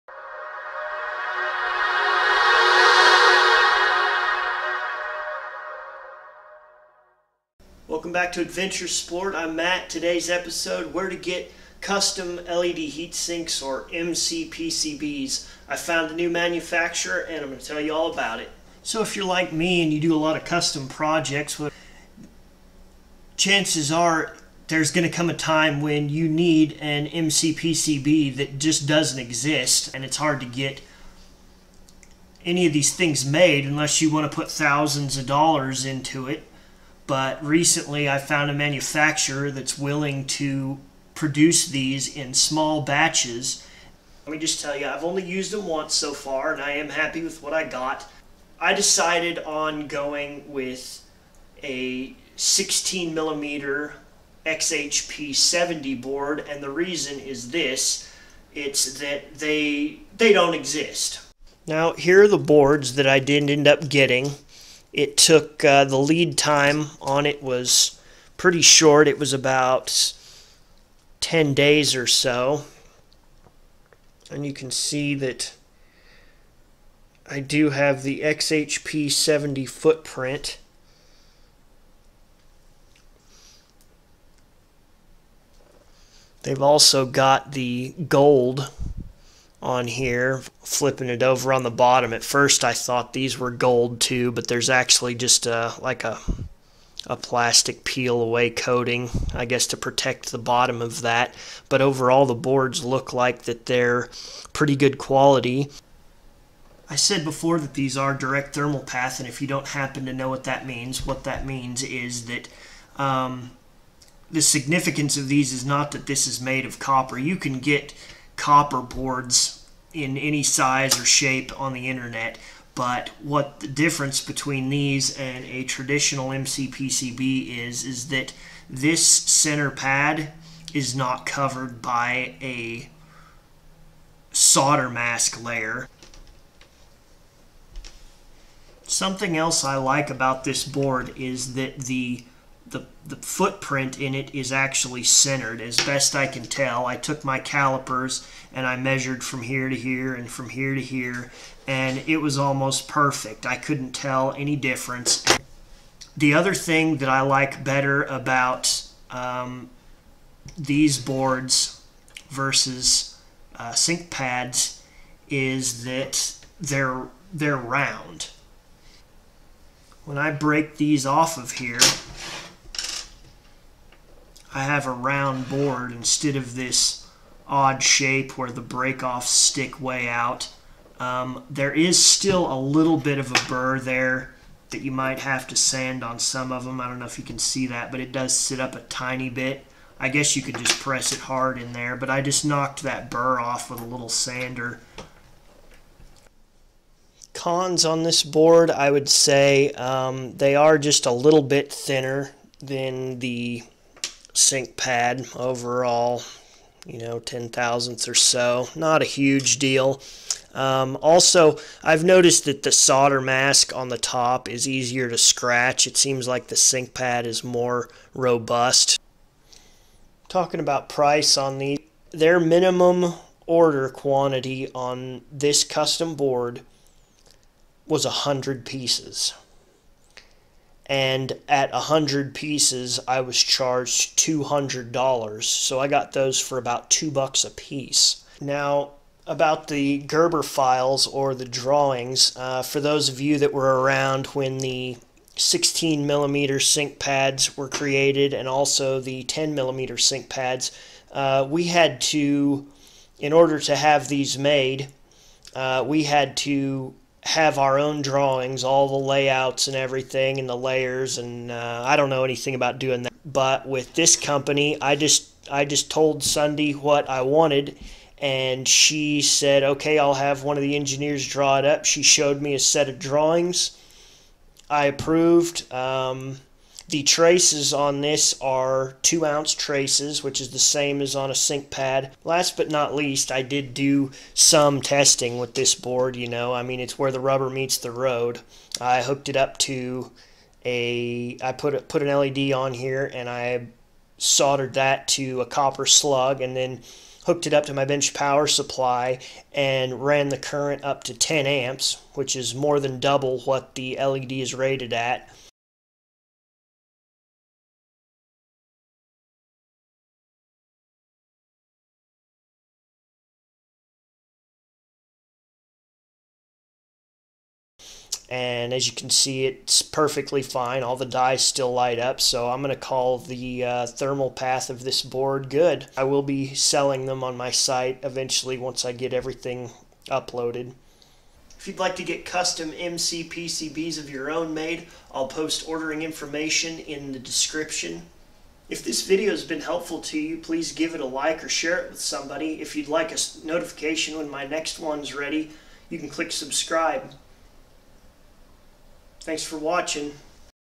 High Power LED - LED Heat sink - Custom Made to Order!
Need custom LED heatsinks for high power LEDs? Now you can get them made in small batches.  Copper direct thermal path mcpcbs.

My contact person was Sundy